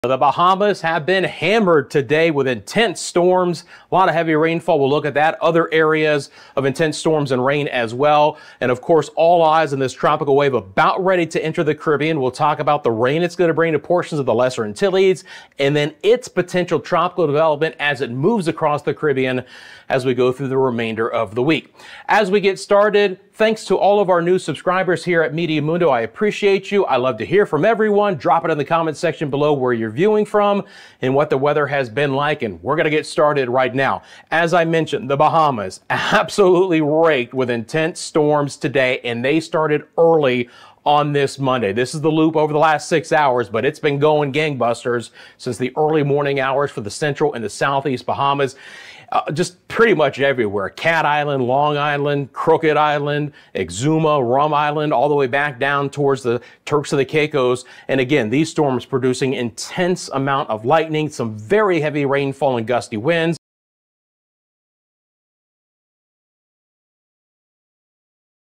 Intense Storms Hit Bahamas and Caribbean | Tropical Development Forecast | Meteo Mundo
Intense storms hitting The Bahamas, along with heavy rainfall in other areas. A tropical wave entering the Caribbean, bringing rain to the Lesser Antilles and potentially developing into a tropical system.